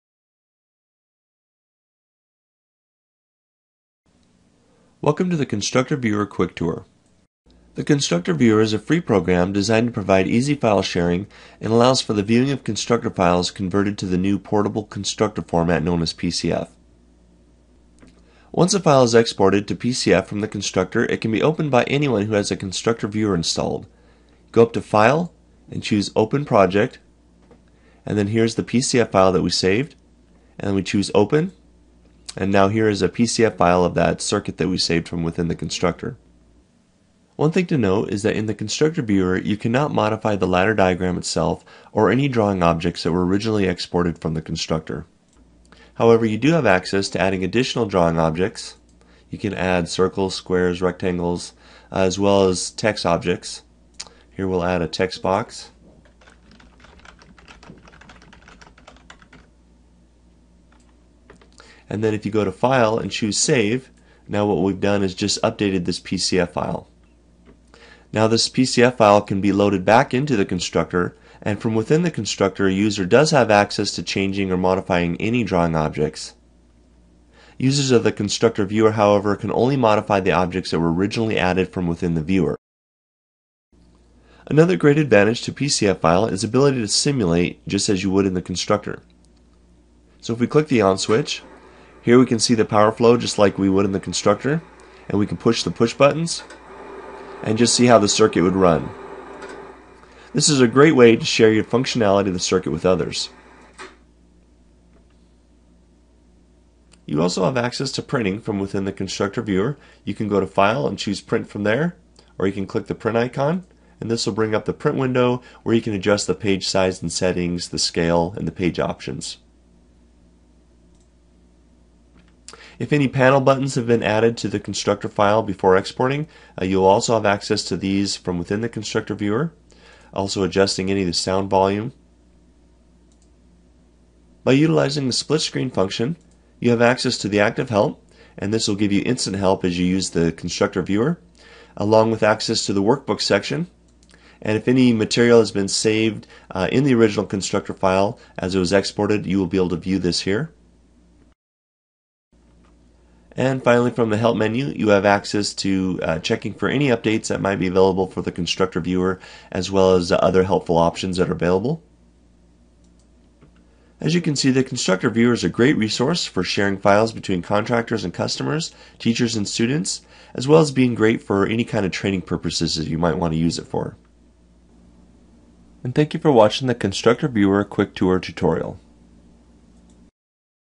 Constructor Viewer Quick Tour
A quick run through of the free Constructor Viewer which allows you to view Portable Constructor Format (PCF) files exported from the Constructor. View, print and simulate just as you would in the Constructor.